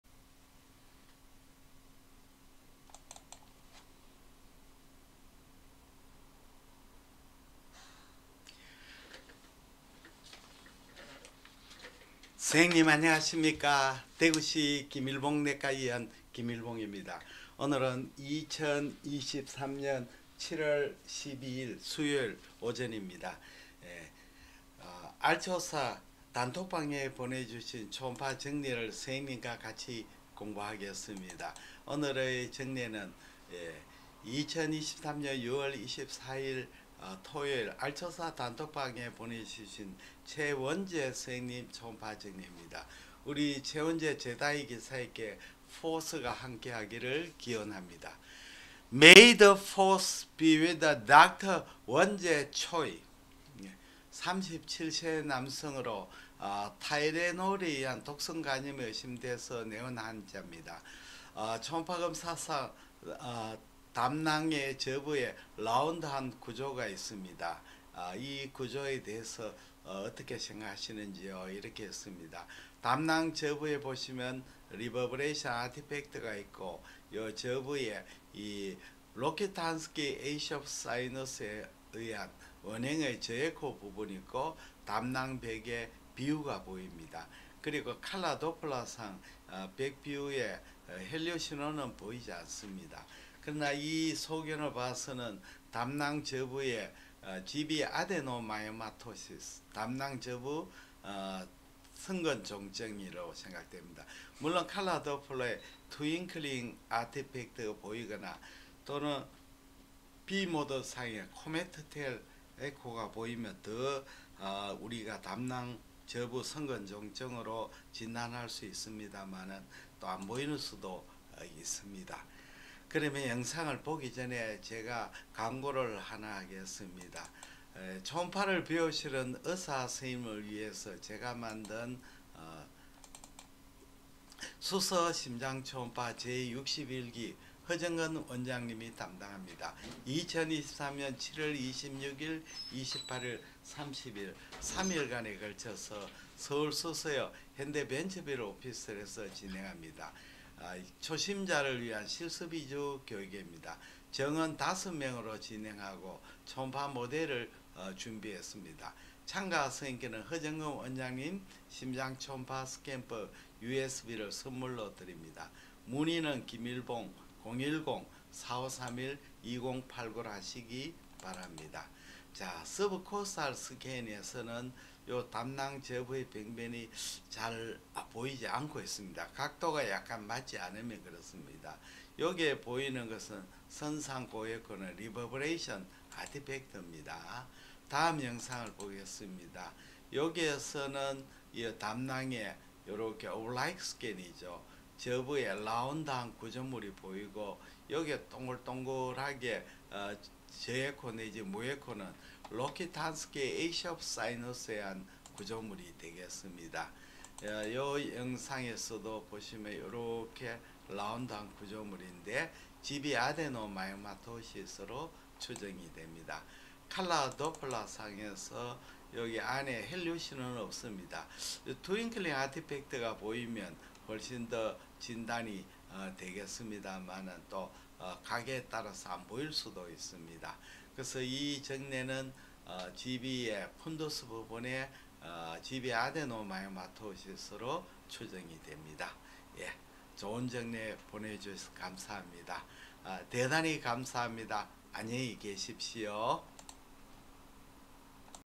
초음파 증례 토의(최원재) 2023년6월24일 담낭 저부 선근종증
RAS로 추정되는 다발성  저에코 부위를 동반한 담낭 저부 벽비후를 보이는 담낭 저부 선근종증, 37세 남성